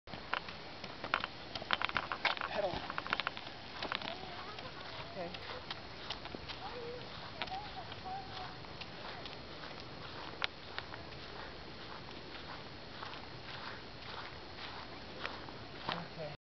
Nolan learning to ride his bike
We rode up and down that hill lots of times, and he got a little better each time.  It reminds me of the day he took his first steps.  Pretty cool.